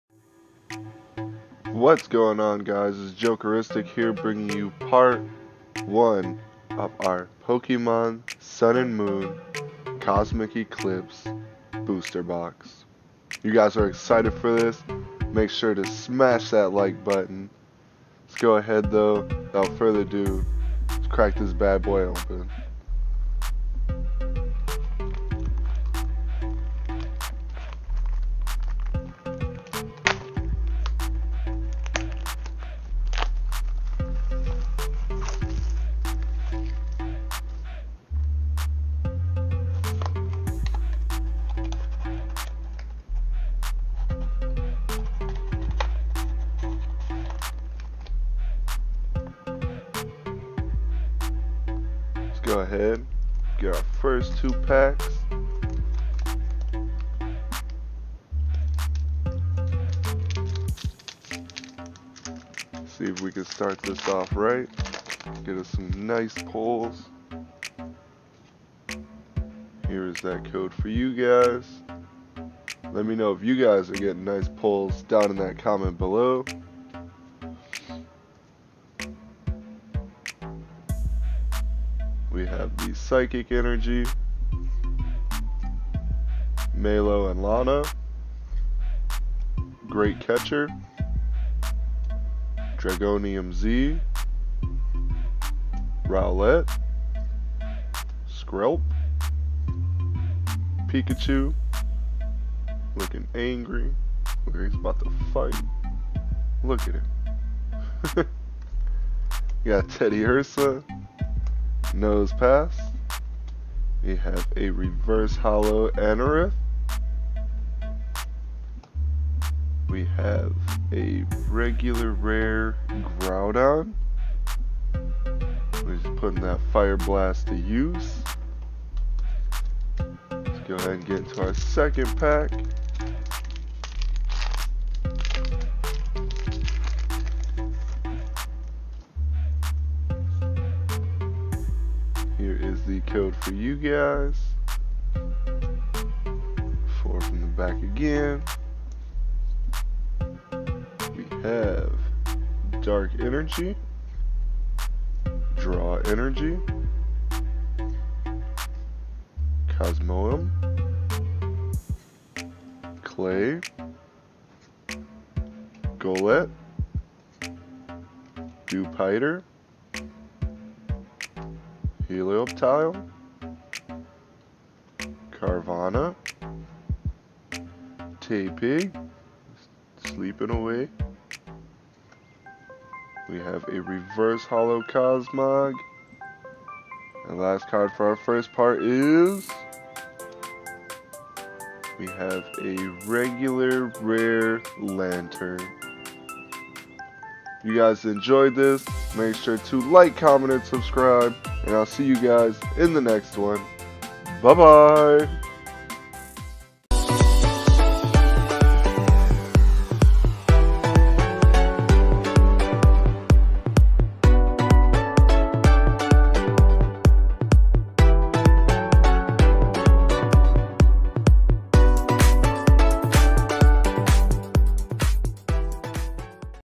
Pokemon Sun & Moon Cosmic Eclipse Booster Box Opening Part #1 With Jokeristic
This Is Part 1 Of our Pokemon Sun & Moon Cosmic Eclipse Booster Box Opening. lets see what pulls we can get from these packs and remember if you guys enjoy this make sure to like, comment, and subscribe and ill see you guys in the next one BYE BYE!

Playlist For Unified Minds Booster Box